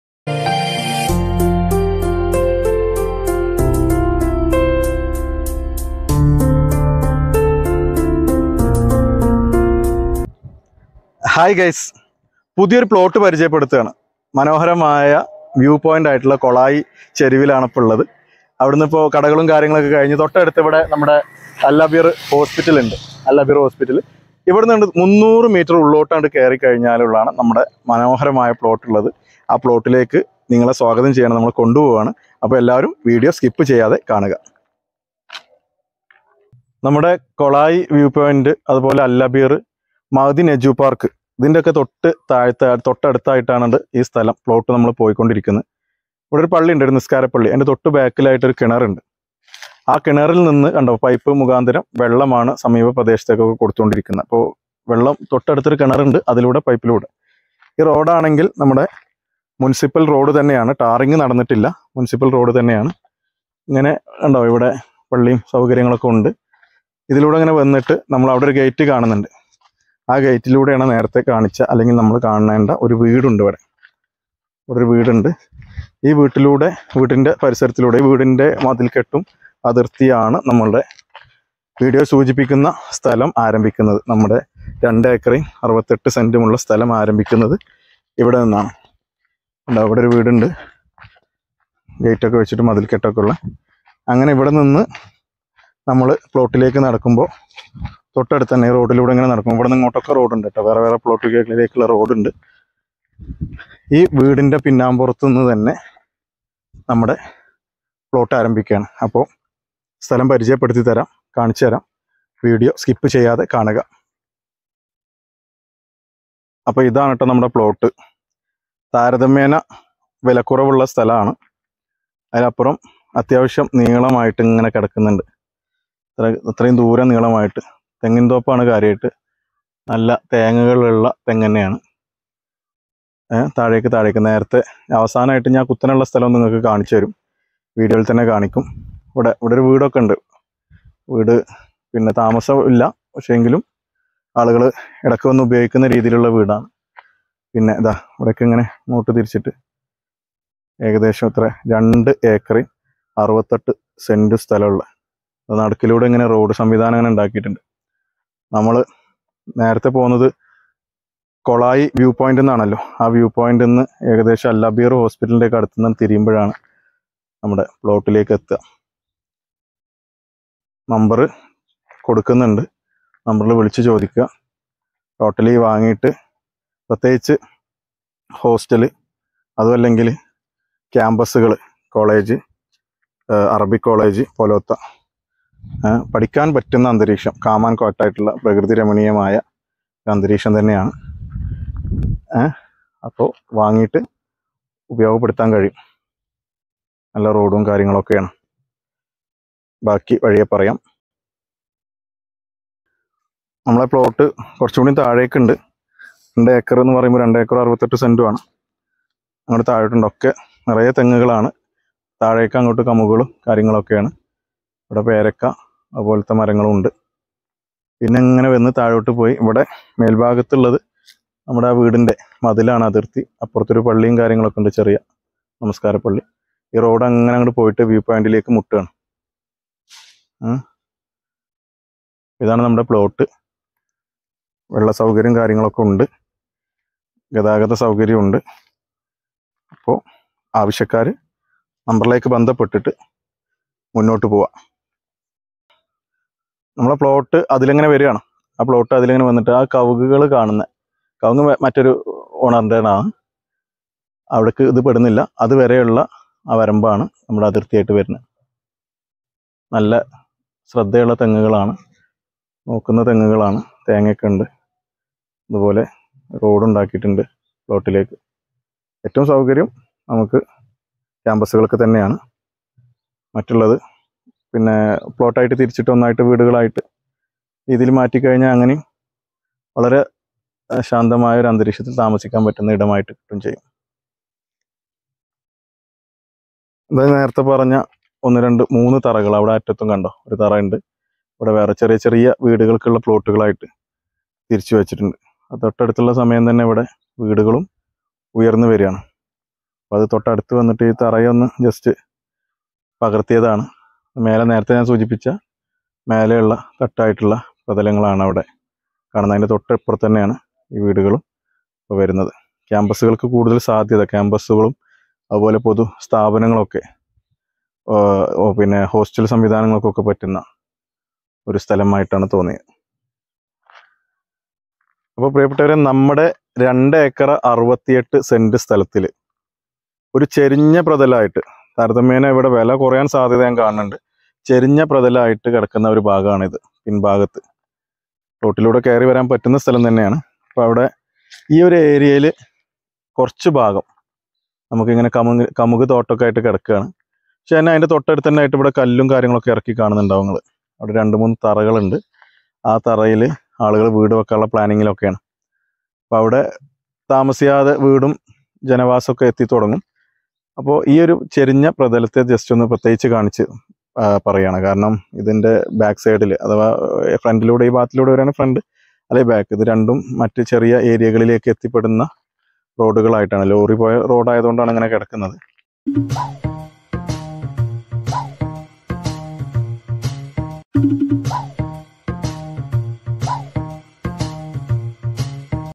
മലപ്പുറം കോളായിയിൽ നിന്നും 300 മീറ്റർ ഉള്ളിൽ 2 ഏക്കർ 68 സെന്റ് സ്ഥലം സ്ഥാപനങ്ങൾക്ക് ഉചിതമായത് - V5
മലപ്പുറം കോളായിയിൽ നിന്നും വെറും 300 മീറ്റർ ഉള്ളിൽ 2 ഏക്കർ 68 സെന്റ് സ്ഥലം സ്ഥാപനങ്ങൾക്ക് ഉചിതമായത്